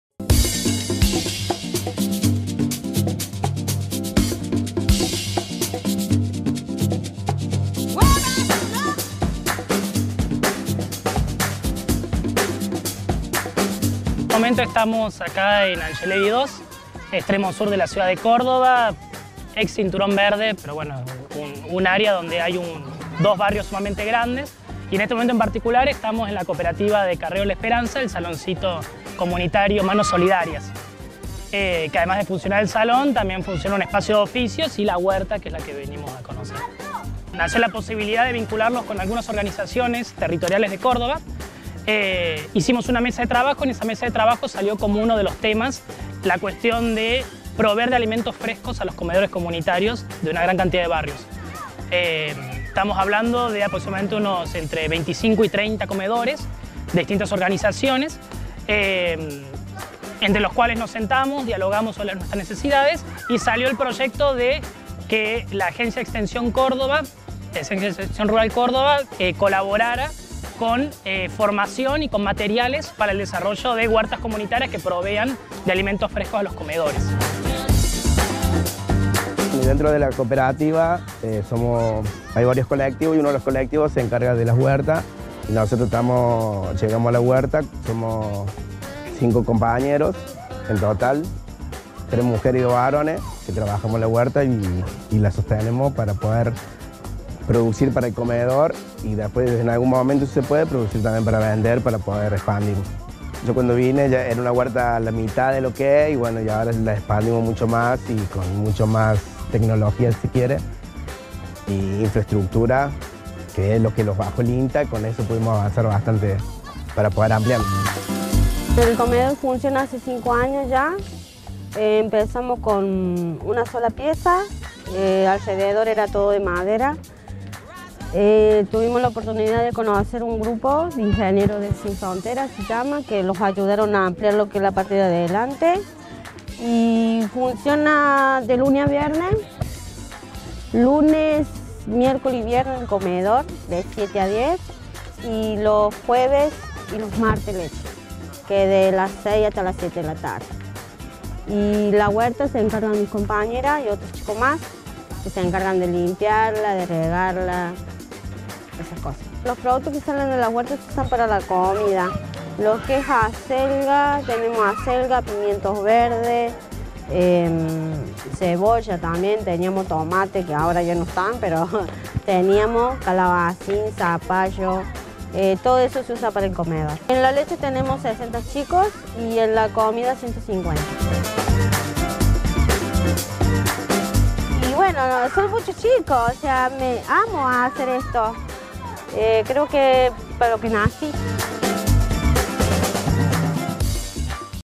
INTA Manfredi - ProHuerta en comedores comunitarios
En la ciudad de Córdoba el INTA junto con el Ministerio de Desarrollo Social de la Nación fortalecen el trabajo de comedores comunitarios a través de la compra de insumos y producción de alimentos frescos.